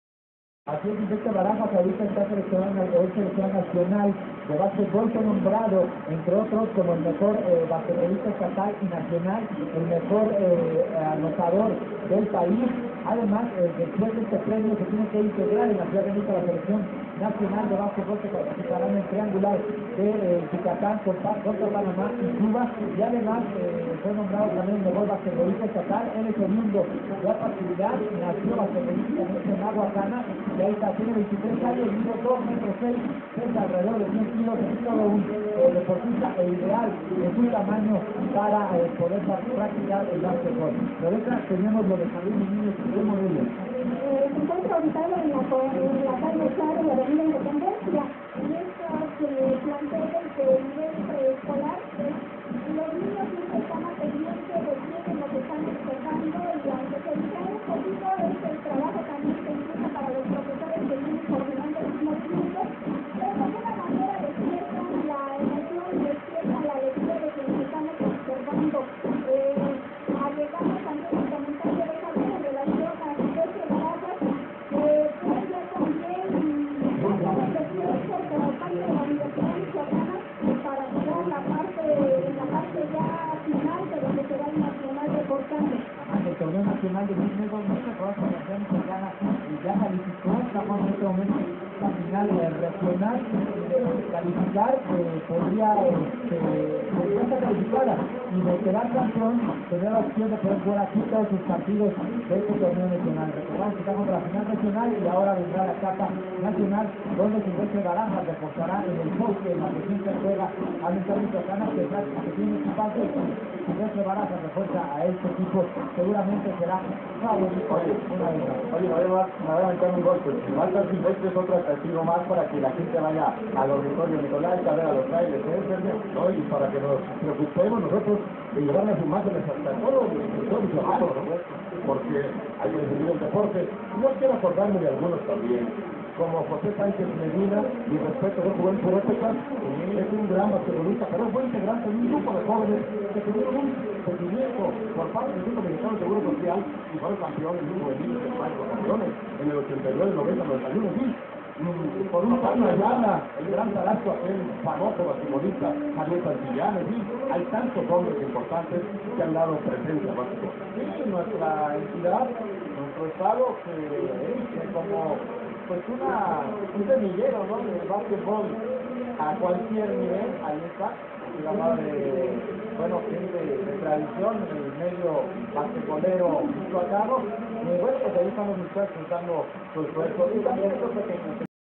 sistema michoacano de radio y television
20 de noviembre de 1997,transmision del desfile por el smrtv